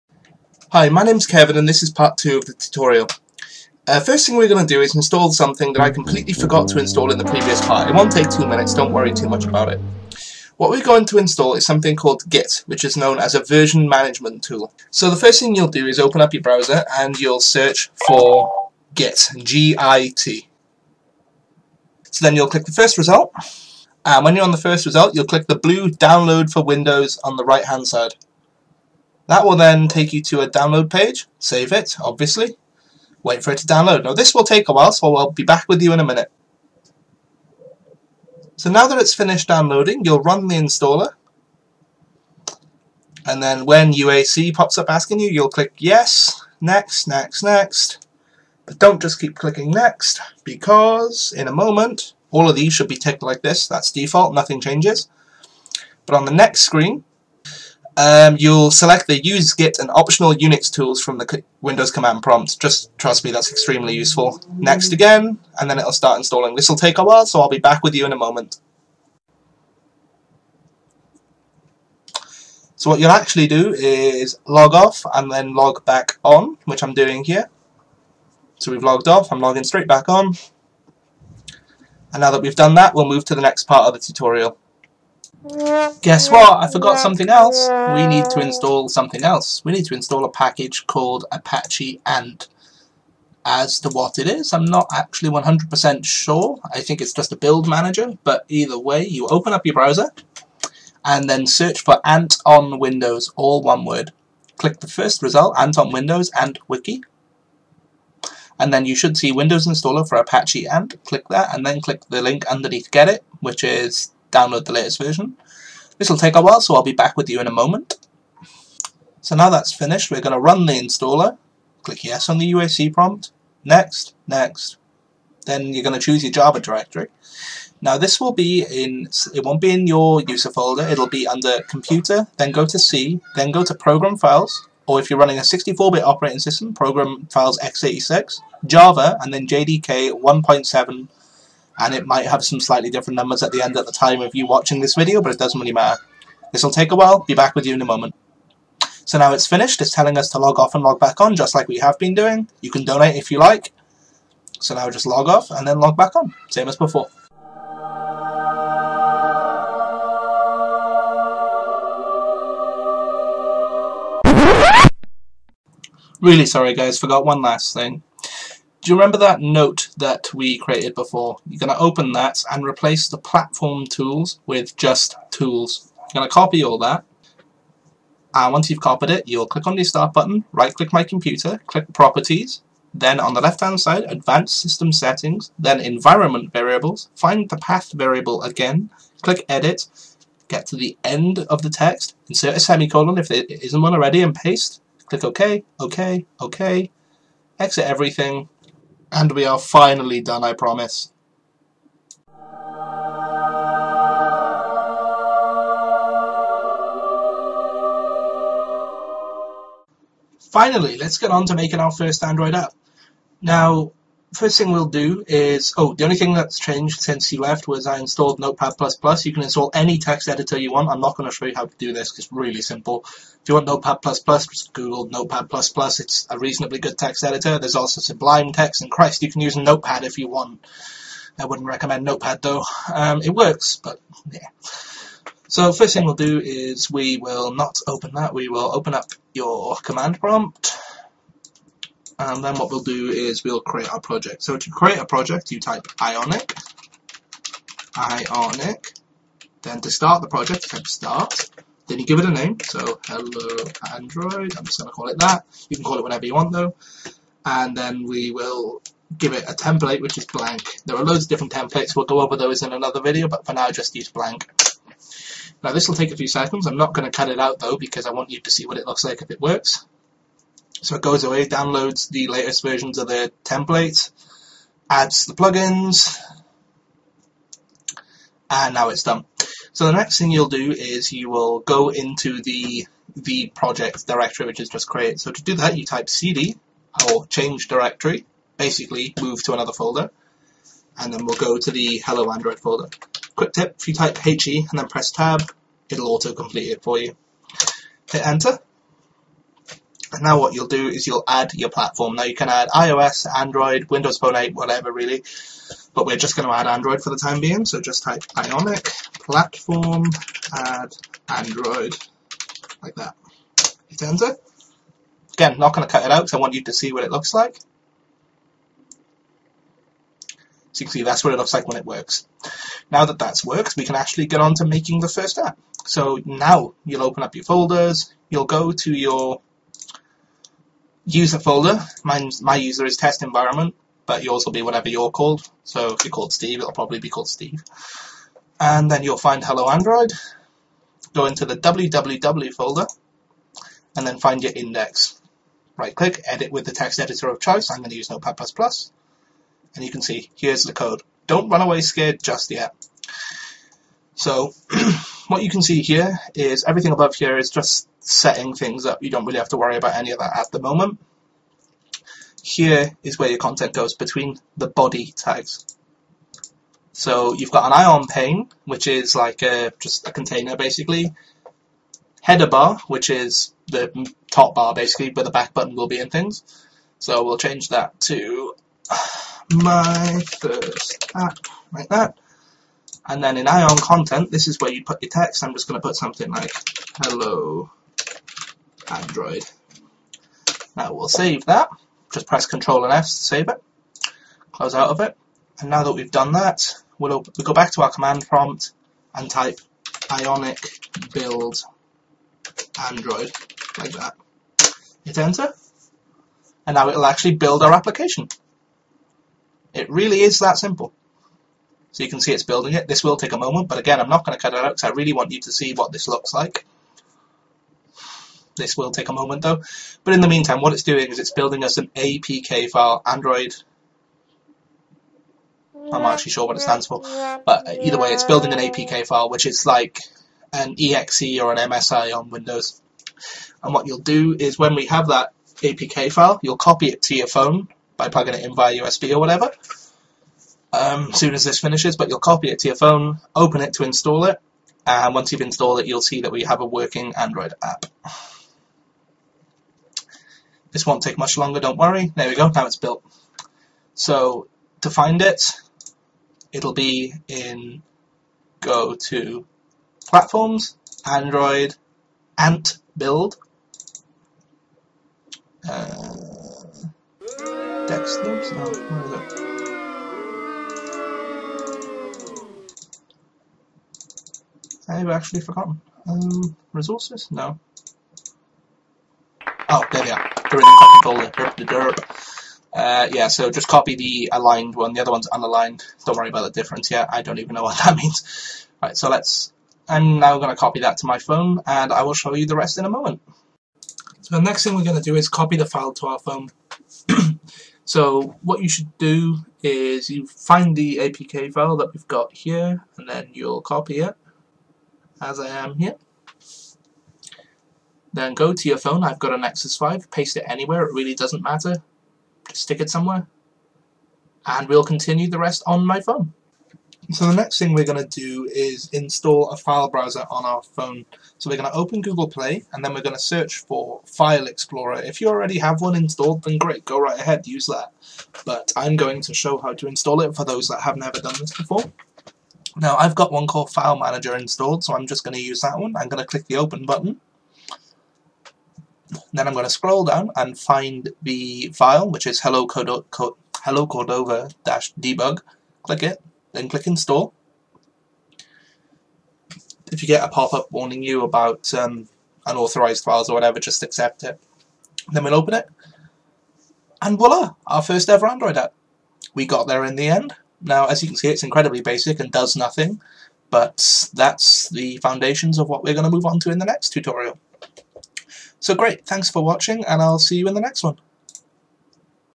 Cordova and Ionic | Android HTML5 App Development Tutorial | Part 2
Hey guys
We are going to create an Android app using pure HTML5/CSS3 using the Ionic Framework and Cordova.

In this part we will actually get to create a very basic but working Android application, not before clearing up some stuff I forgot to mention in the previous tutorial. Doesn't take long don't worry!

The app we create will work fully on Apple iOS as well as Windows phone 8 but we will focus on Android in this tutorial.

This tutorial will work best on Windows 7 and up, but everything discussed later will work on Mac and Linux, But the installation procedure will differ greatly.